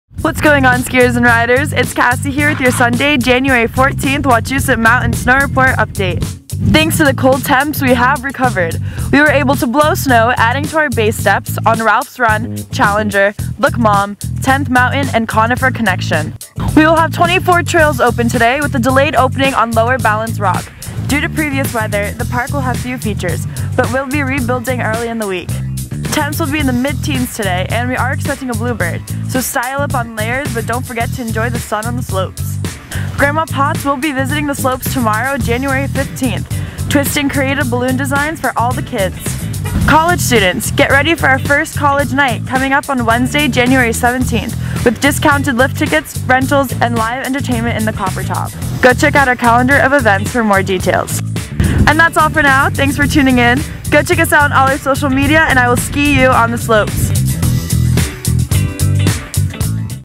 Jan. 14, 2018 Wachusett Mountain Snow Report
Come enjoy the runs you've been waiting for, we've made snow!